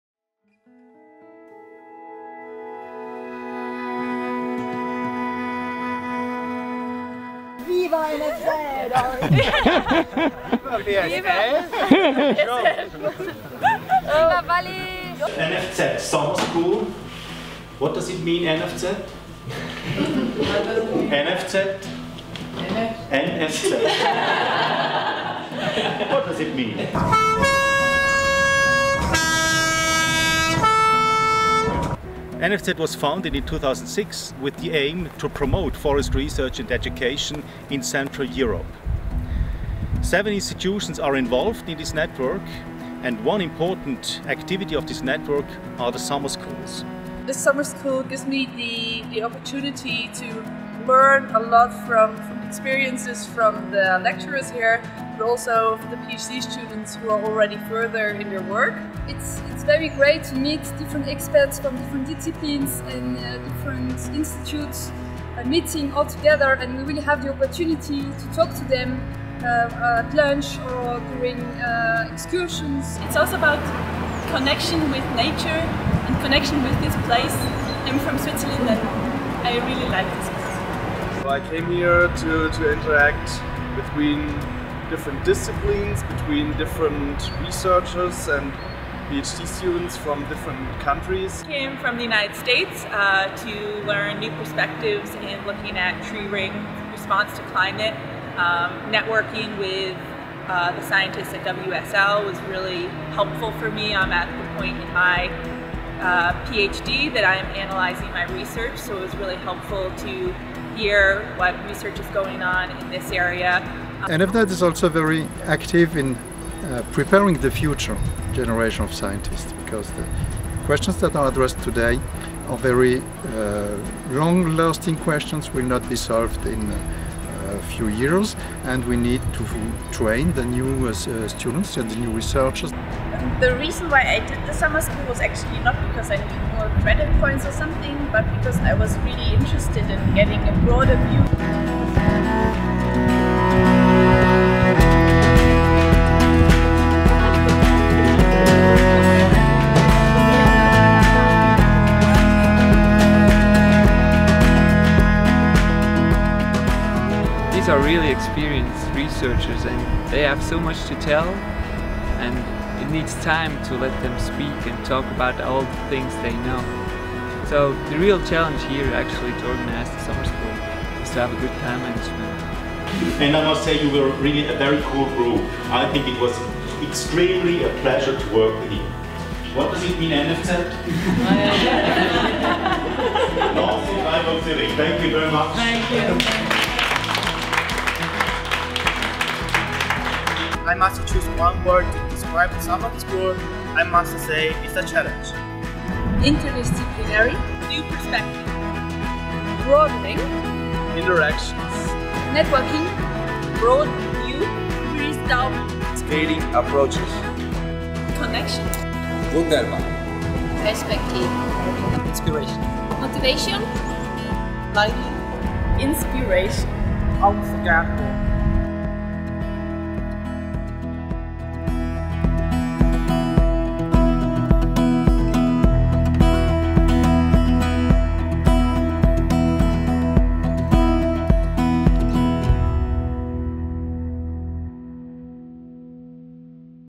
NFZ Summer Schools: Smart and Fun
NFZ.forestnet is a European research and higher education network which gathers 7 French, German and Swiss institutions working on forestry, forest ecosystem and forest-related products. The summer school programme was the first project to be developed within NFZ.forestnet. NFZ summer schools are alternately organized each year by one of the three partner countries. The 2015 summer school took place in Fafleralp (Valais), in the Swiss Alps.